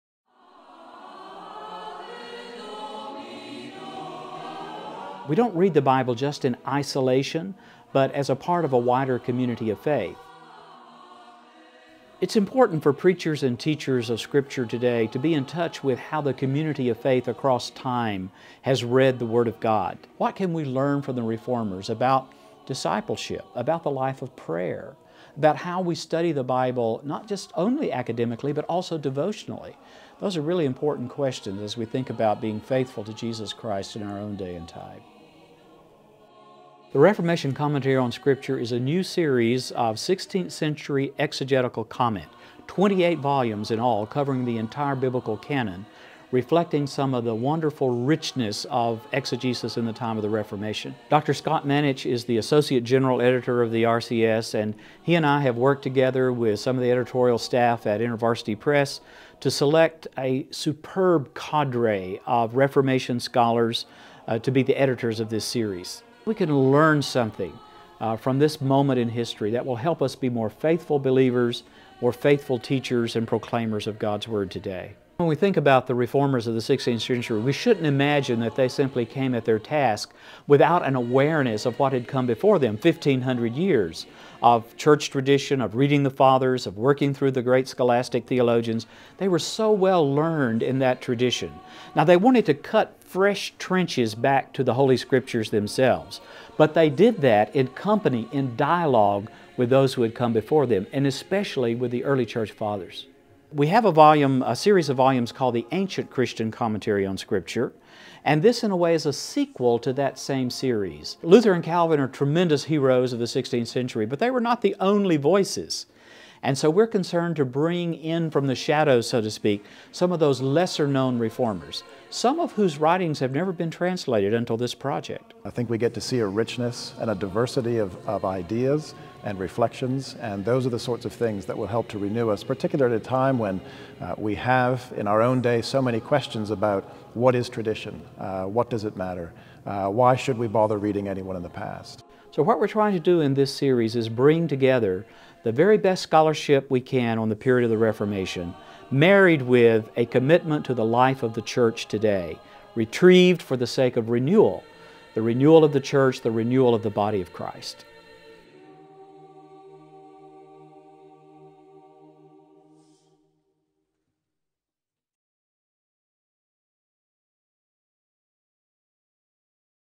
The Reformation Commentary on Scripture
The Reformation Commentary on Scripture (RCS) follows an ancient practice of biblical commentary, in which the scriptural texts are elucidated by chains of passages collected from the authoritative insights of the churchÕs great exegetes. Each volume consists of the collected comments and wisdom of the Reformers collated around the text of the Bible. Here is a unique tool for the spiritual and theological reading of Scripture and a vital help for teaching and preaching.